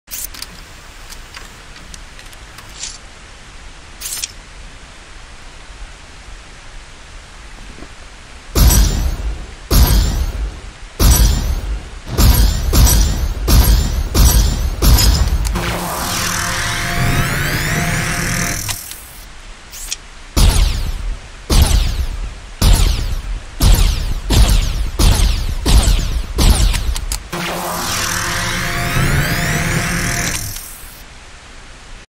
Aftertouch Weekly Challenge 17 2022
Design gun fire sequence and foley for the following clip.